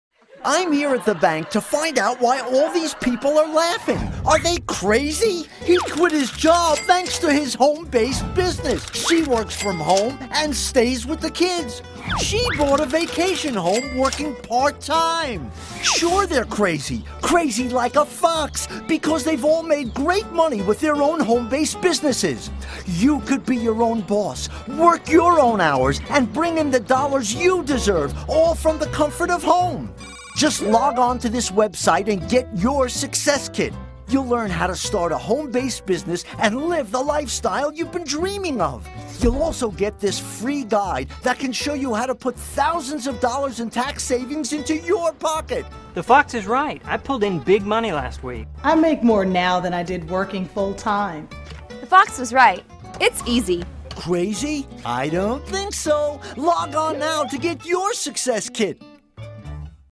Crazy Fox - Multi Level Marketing (MLM) scam
You can read my article about this nonsense titled:

How Multi Level Marketing is a great way to piss off your friends and ALWAYS leave you poor